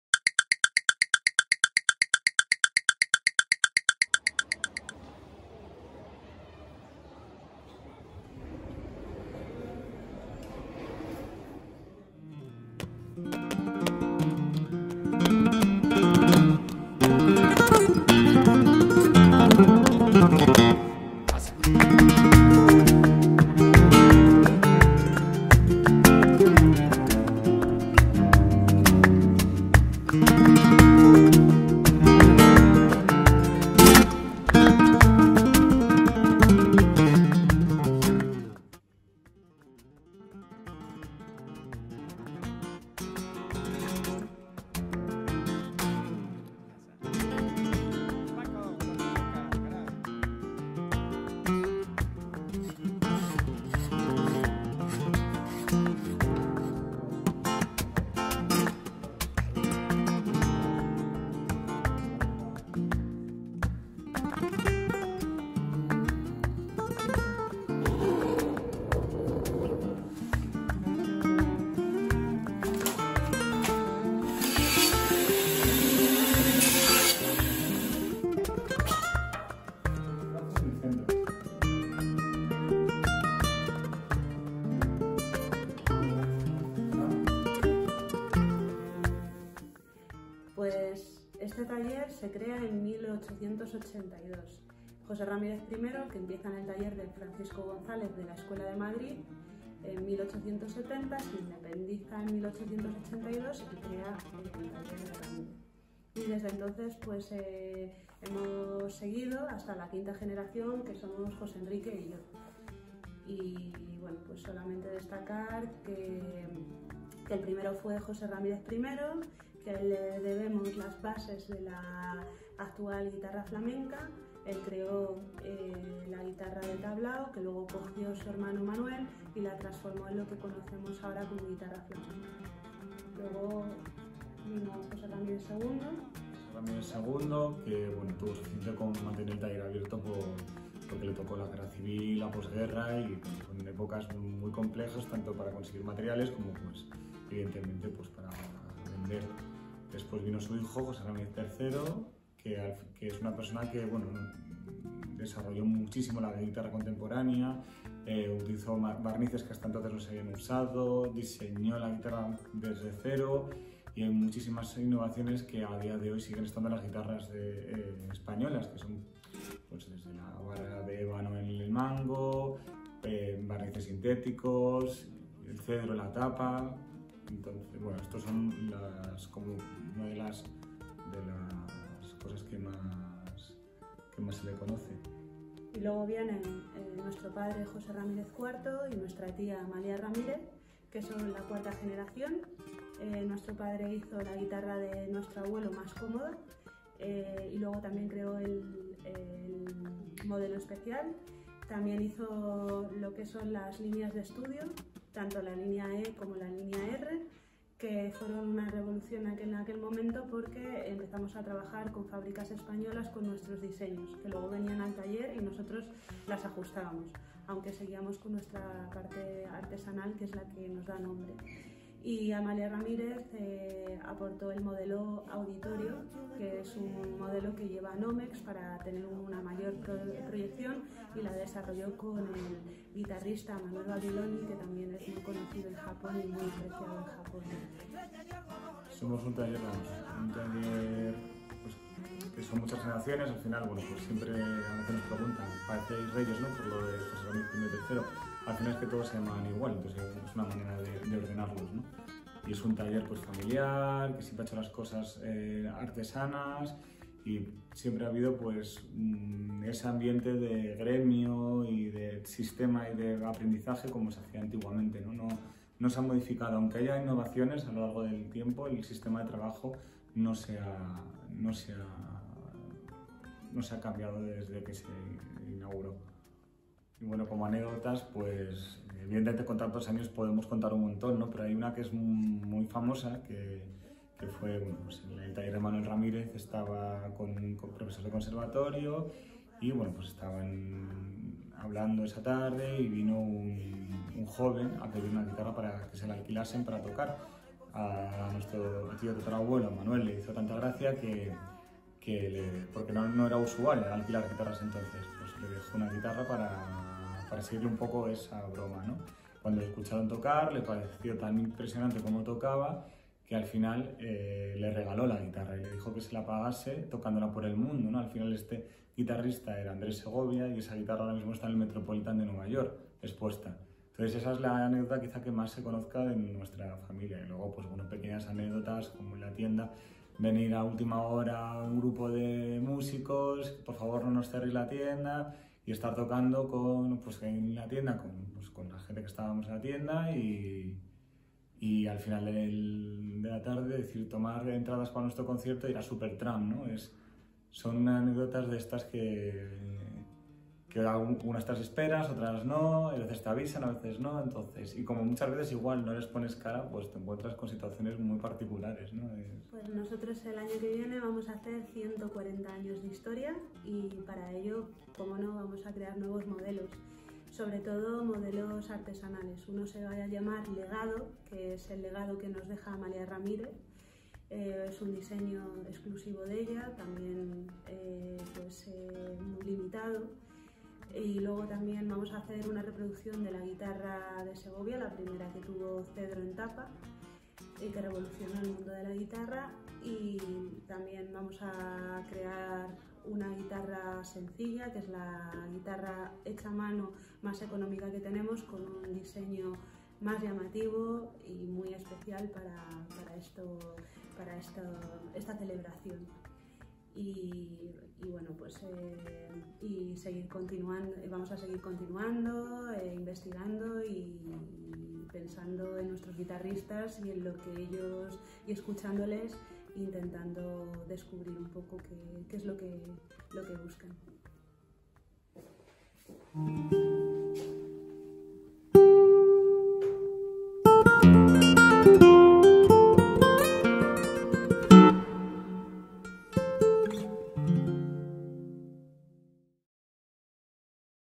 RAMÍREZ GUITARS FOR JAPAN
(🇬🇧⬇️) Gracias a la camaradecomerciodemadrid y a Mirai Kobayashi por este vídeo para Japón. La música es de nuestro amigo @pacovidal. ¡Feliz año y feliz noche de reyes!, empezamos con buen pie 😉
(🇬🇧) Thanks to @camaradecomerciodemadrid and Mirai Kobayashi for this video for Japan. Music is by our friend @pacovidal Happy new year, we're on to a great start 😉.